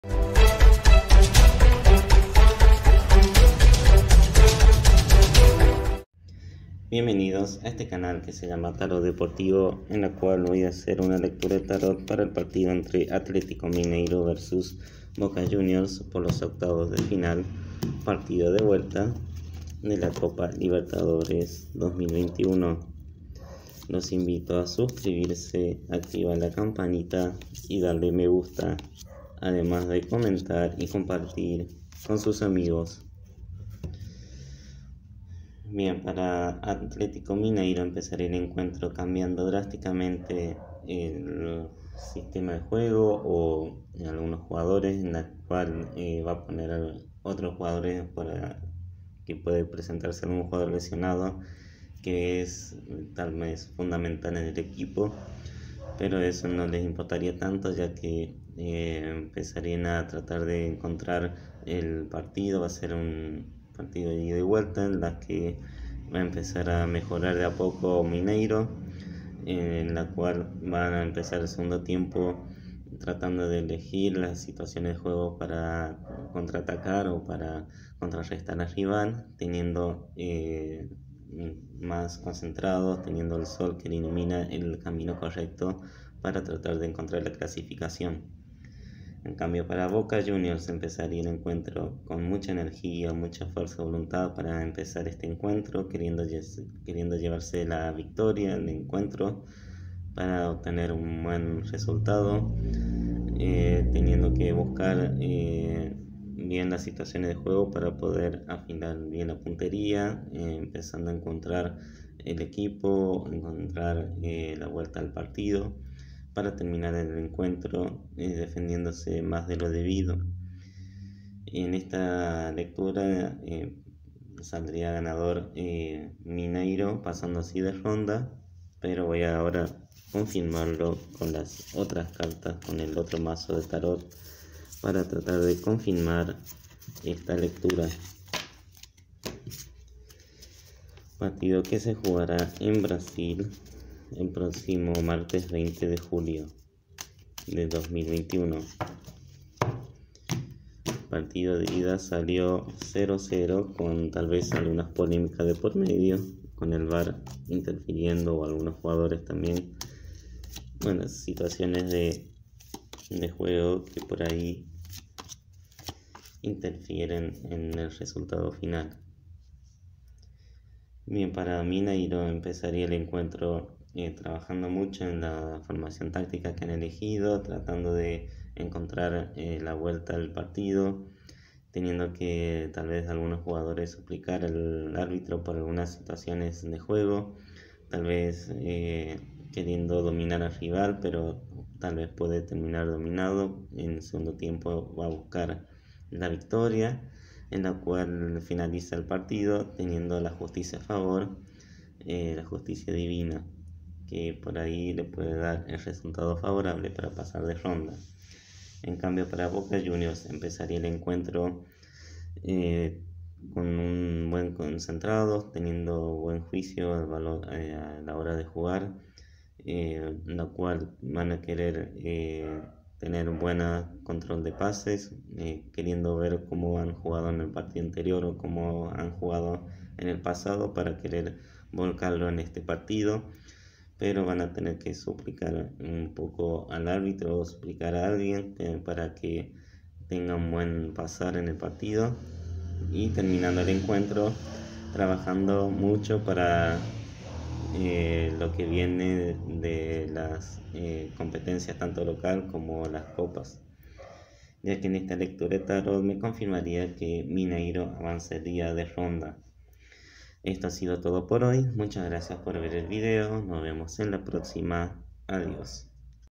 Predicción Atlético Mineiro vs Boca Juniors Octavos de Final Partido Vuelta Copa Libertadores 2021
Tirada de tarot con cartas Rider y Egipcias para el partido correspondiente a los octavos de final partido de vuelta de la Copa Libertadores 2021 que se jugará este próximo martes 20 de julio del 2021